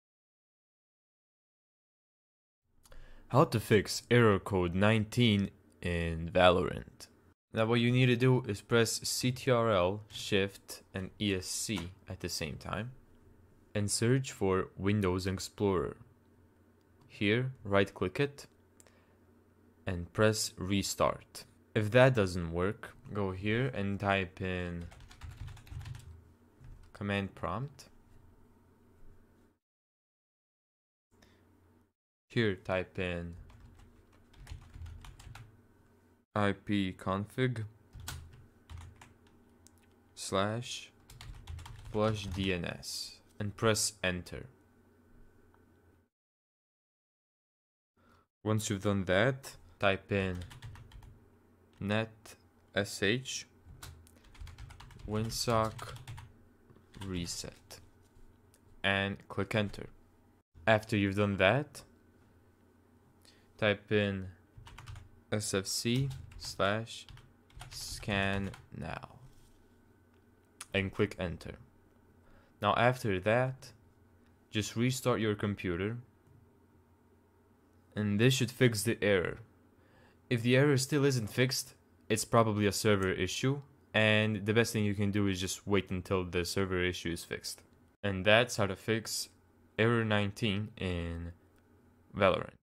How To Fix Error Code : VAL 19 In Valorant
There was an error connecting to the platform. Please restart your client. Error Code: VAL 19. If that error messages seems familiar to you then you've come to the right video, learn how to fix error code 19 in Valorant.